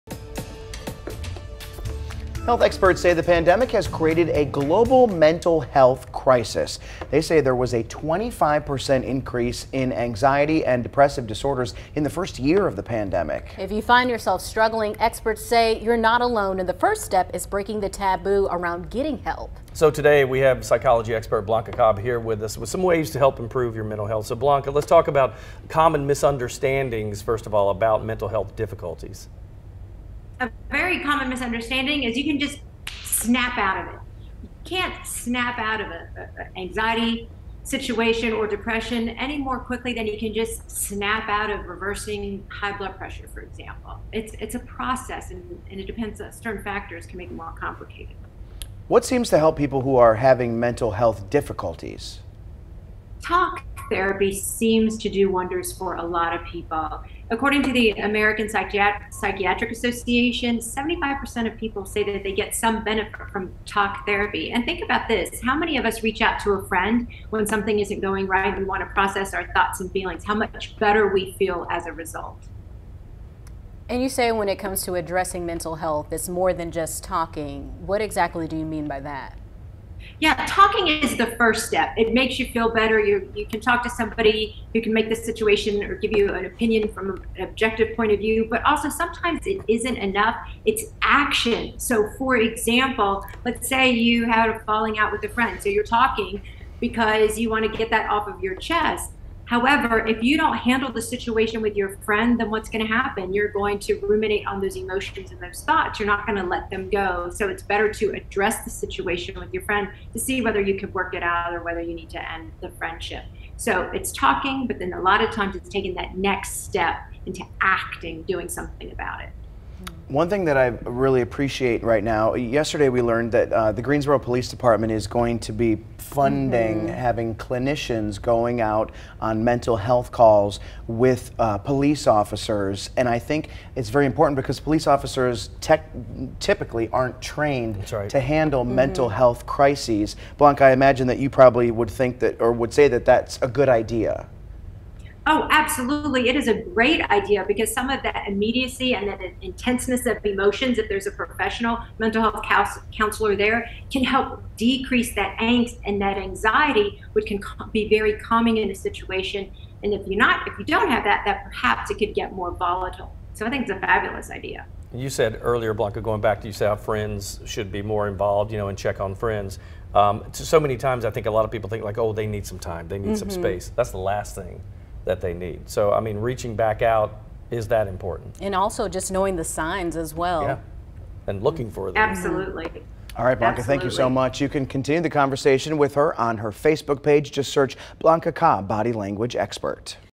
How to improve your mental health
Health experts said there was a 25% increase in anxiety and depressive disorders in the first year of the COVID-19 pandemic.